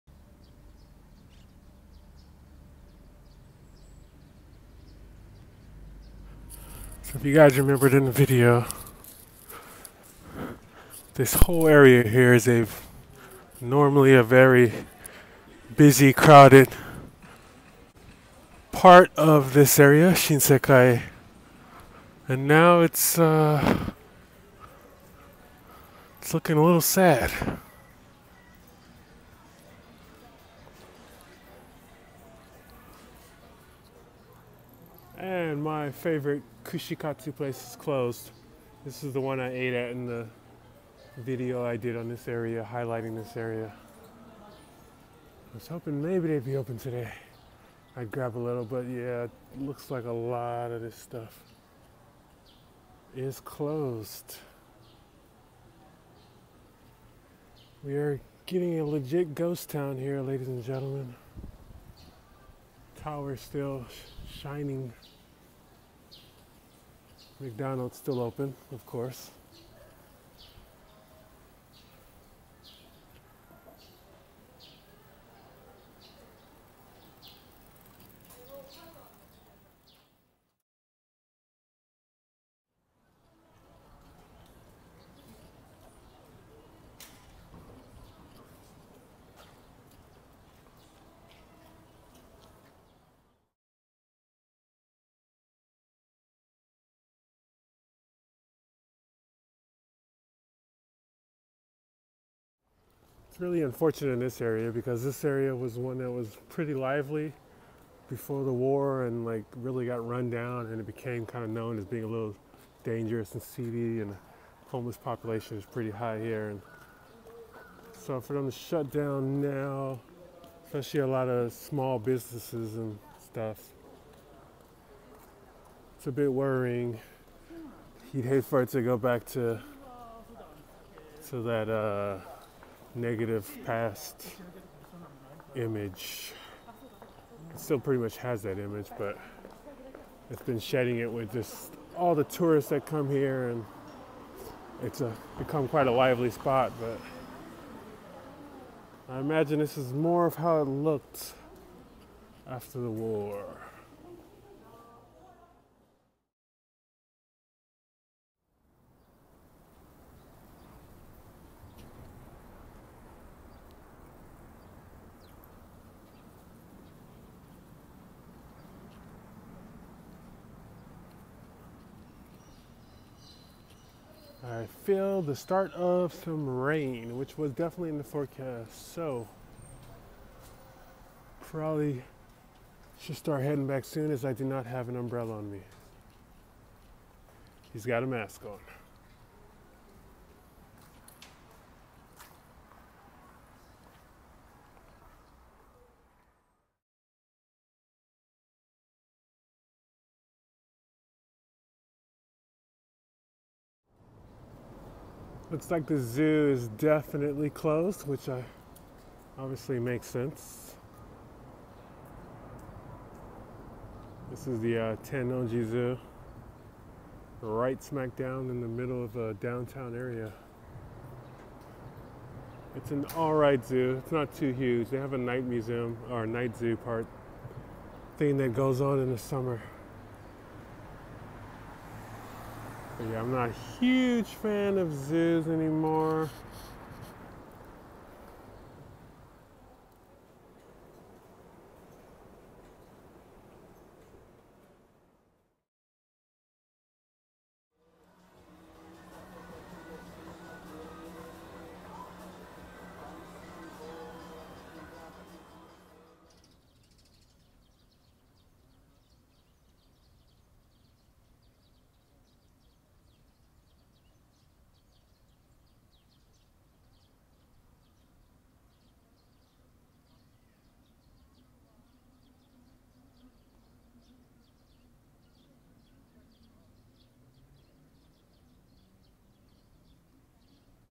Osaka’s usually lively Shinsekai during COVID
Put together a quick little video to show how one of Osaka's most liveliest areas looks during this COVID crisis. As Japan isn't on any official lock down a few people are still out and about but nothing compared to the numbers this places usually gets particularly as a popular place for tourists. To see just how lively and cool this place can be check out the link to a video I made last year before all the madness.